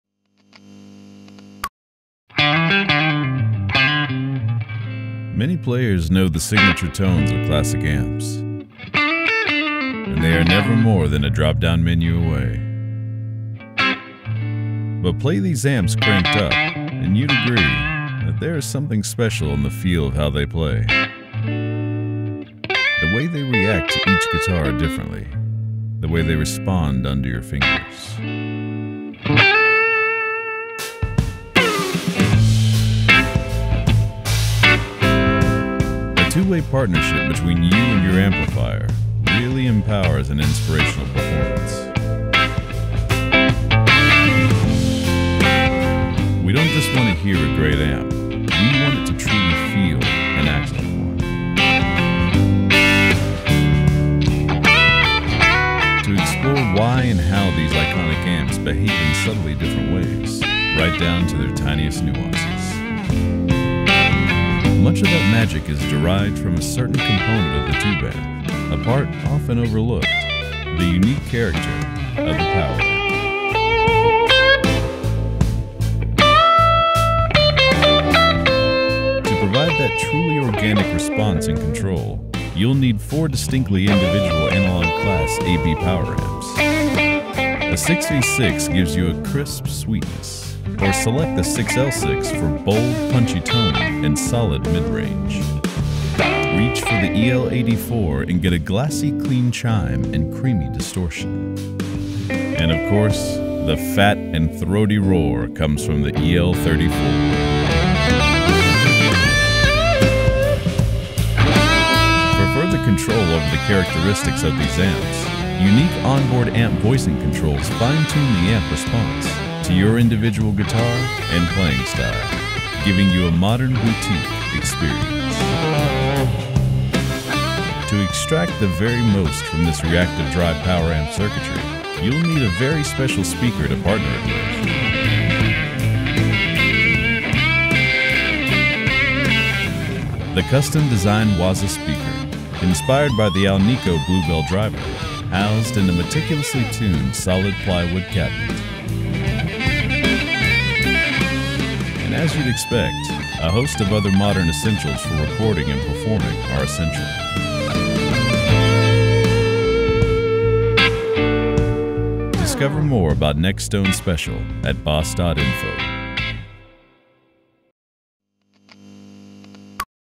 BOSS Nextone Special - A Modern Boutique Experience for Serious Player
BOSS Nextone Special Guitar Amplifier - A Modern Boutique Experience for Serious Players. Designed for pro stage players and discerning tone aficionados, the Nextone series elevates boutique-grade guitar amplification into the modern era. Backed by Tube Logic, Nextone amps offer sound and feel that rivals the finest tube amps, coupled with distinct advantages for today's players.

If you're serious about your guitar sound, the Nextone series is made just for you.